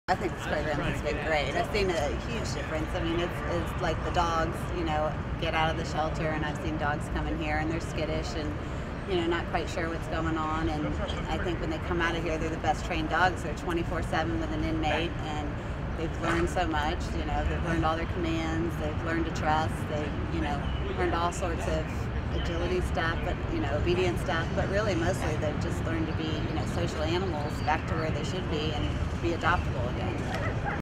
Diane teaches agility and is a big fan of the jail dogs program.
The dogs and inmates always have an awesome time doing agility with Diane.